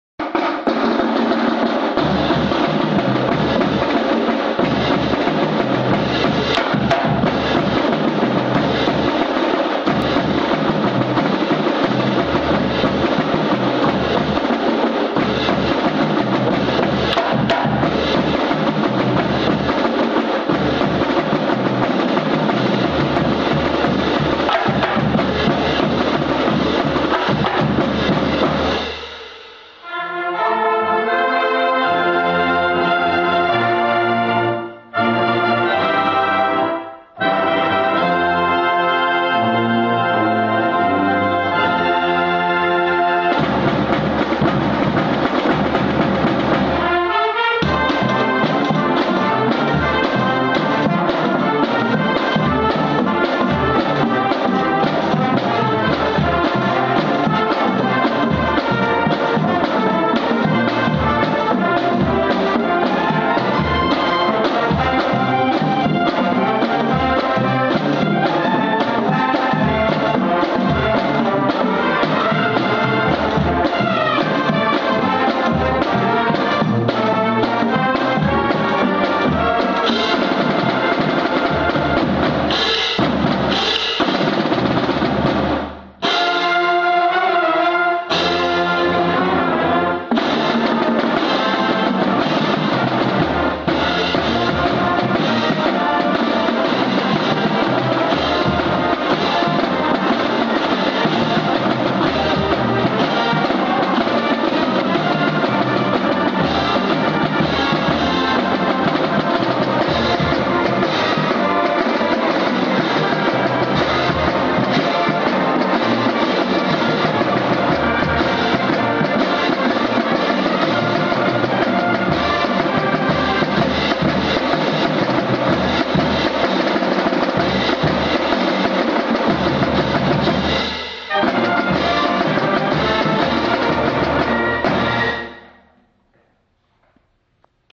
TUMB Band Camp 2011 - Pregame Run
The TUMB rehearsing their pregame sequence during band camp August 2011.